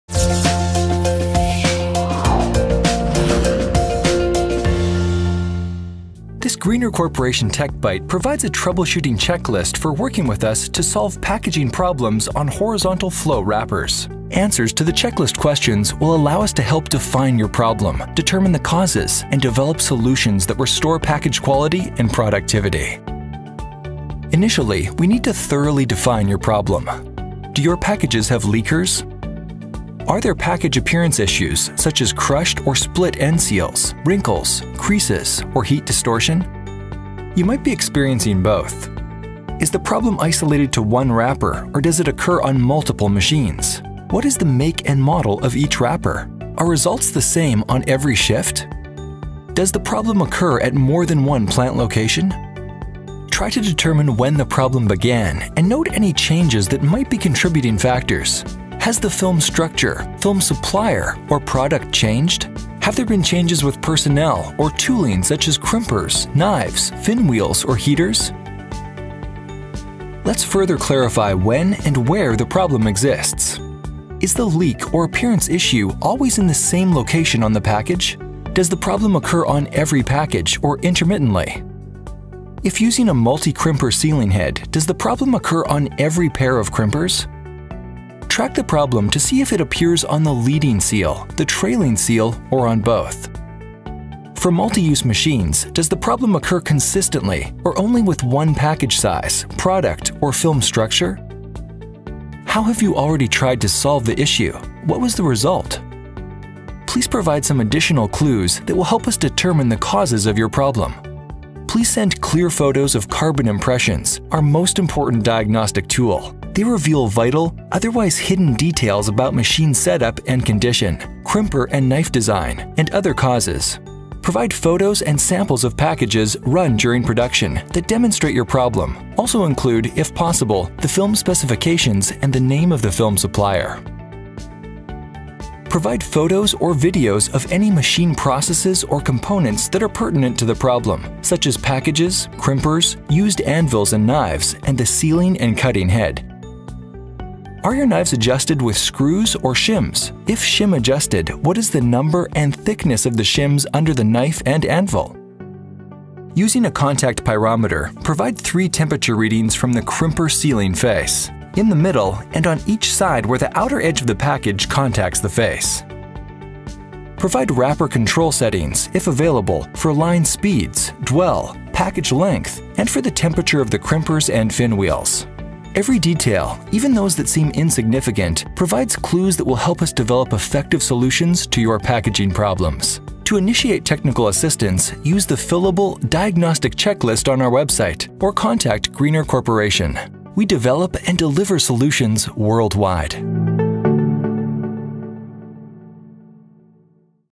Troubleshooting Checklist for Horizontal Flow Wrappers
Español This Greener Corporation "Tech Bite" provides a diagnostic checklist for working with us to solve packaging problems on horizontal flow wrappers. Answers to the checklist questions will allow us to help define your problem, determine the causes, and develop solutions that restore package quality and productivity.

Technical Support is a standard component of all Greener Corporation products and services,
even when travel is restricted.

Read the post and watch the video on our Technical Resource Blog

00:00 Troubleshooting Checklist for Horizontal Flow Wrappers

00:18 Define the Packaging Problem
00:28 Package Leakers and Appearance
00:41 Flow Wrapper Make and Model, shift, and location

00:53 When did the problem begin? What has changed?
01:14 Location and Frequency of Problem on Packages
01:48 Determining Problem Causes – Carbon Impressions
02:03 Provide Sample Packages
02:16 Document Problems with photos and videos

02:28 Knife and anvil adjustments

02:38 Sealing Jaw Temperature Profile
02:48 Flow Wrapper Controller Settings
02:53 Develop Solutions for Packaging Problems